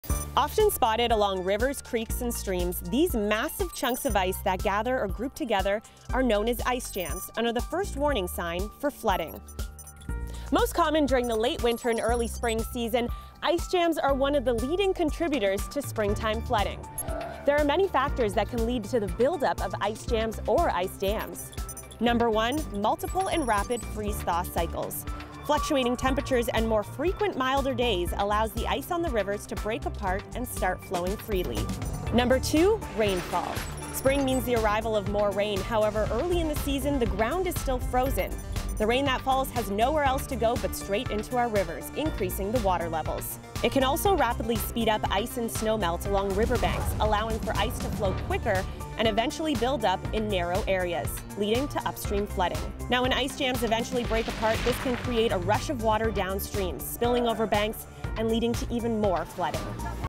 What Causes Ice Jams - The Science Behind the Weather
Meteorologist Kelly Sonnenburg explains the science behind ice jams.